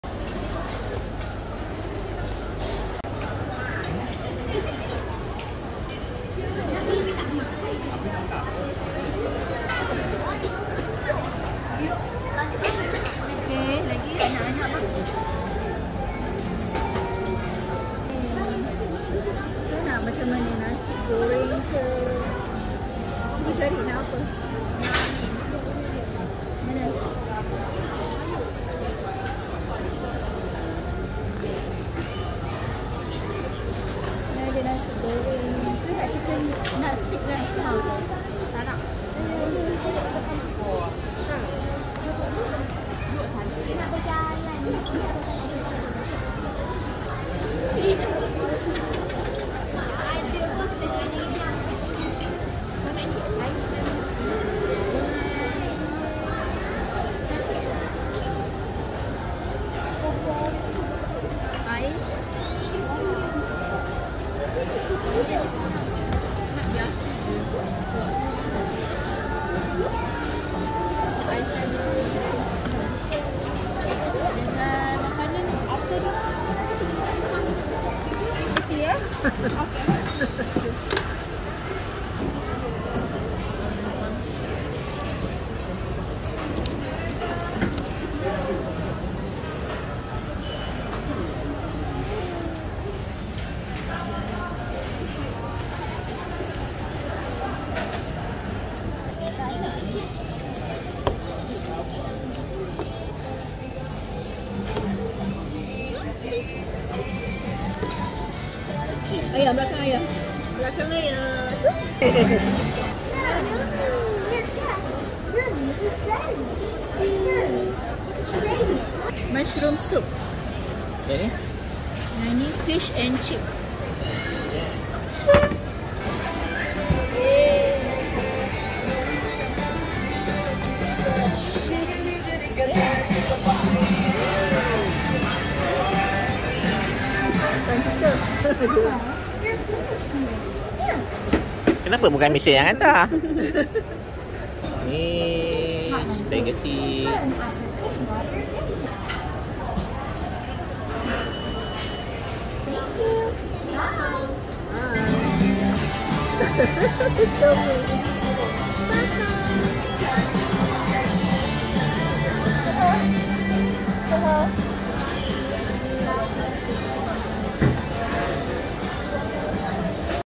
Jangan terkejut jika melihat robot sudah mula ambil alih tugas manusia di restoran // Black Canyon
Setia City Mall Shah Alam